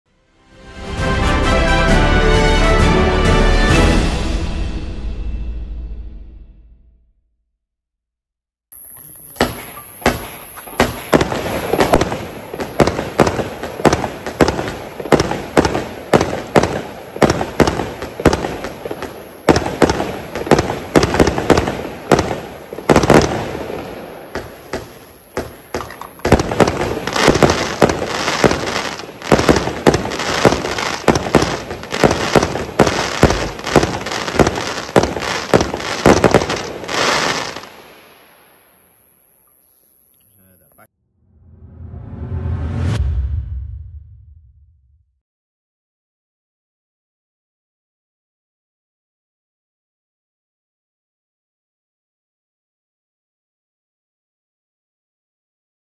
Art. 5116 Perfect Balance Vuurwerkexpert
In deze video zie je de Vuurwerkexpert Perfect Balance art. 5116

Het allerbeste vuurwerk koop je gewoon online via onze webshops. Vuurwerk kopen was nog nooit zo simpel!